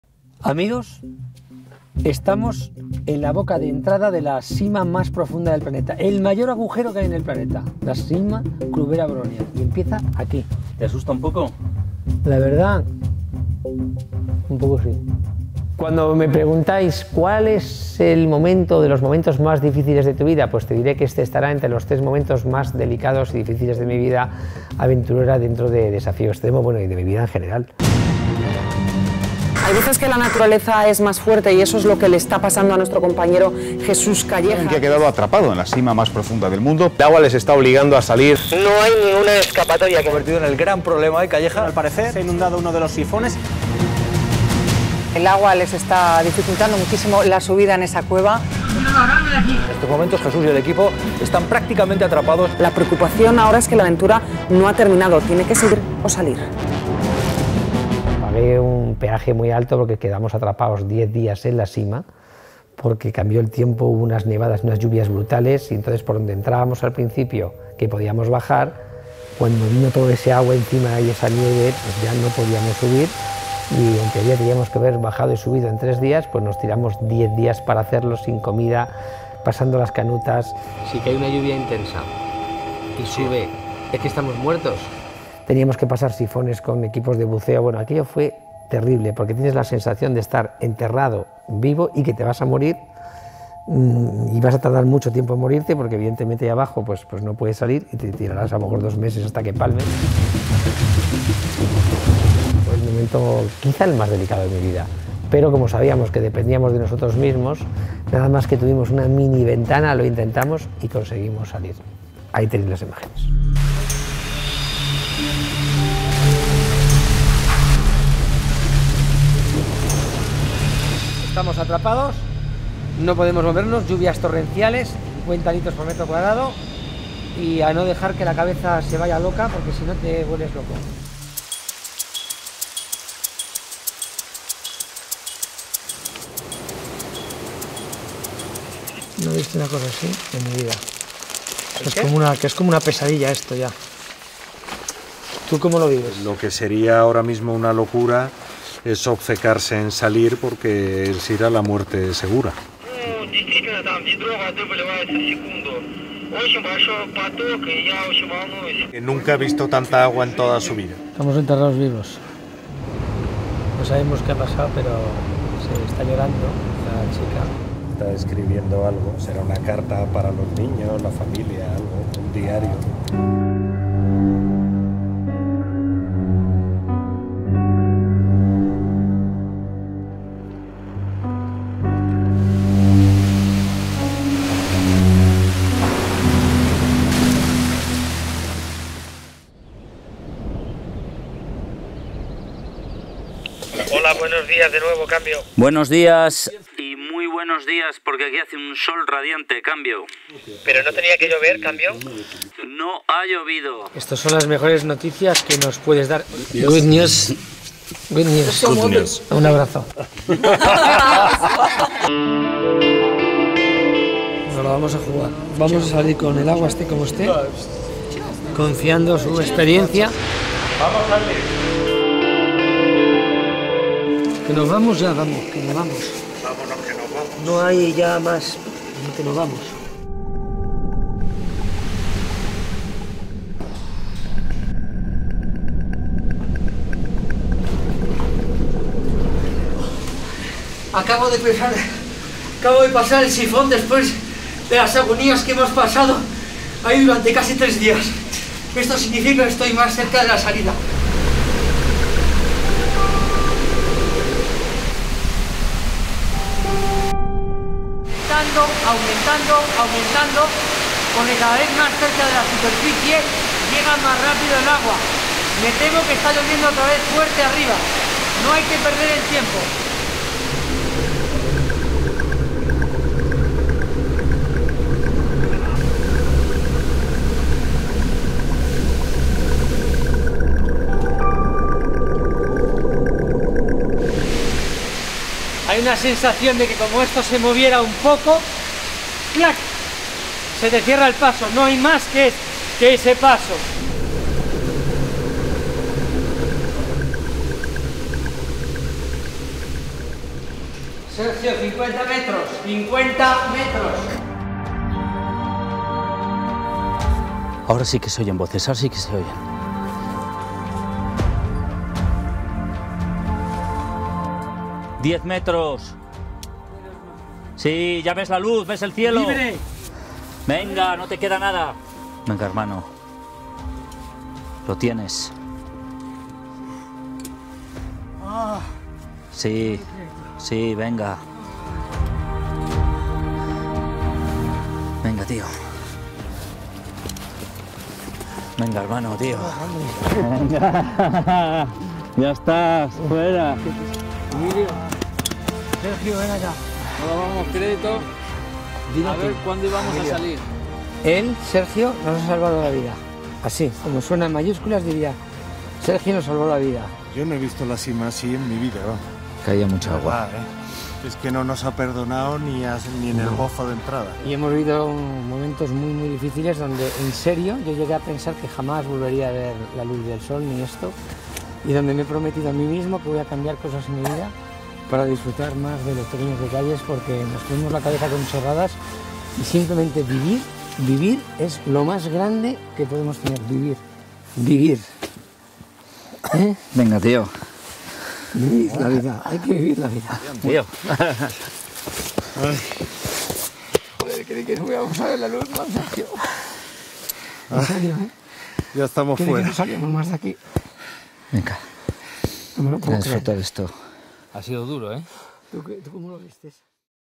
JESÚS CALLEJA | Abjasia, un infierno bajo tierra
Hoy quiero compartir con todos vosotros el momento más duro de mi vida.

Autores: Jose Battaglio/Kaelo Del Rio